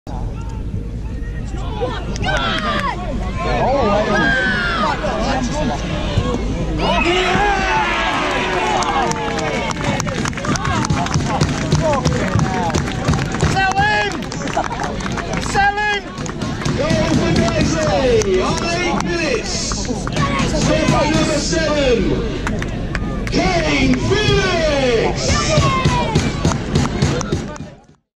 GOALCAM: Kaine Felix opens the scoring for Guiseley against Leeds United
Kaine Felix's strike in the 2-1 pre-season friendly defeat to Leeds United is captured on our goalcam. Footage courtesy of Wainwright Media.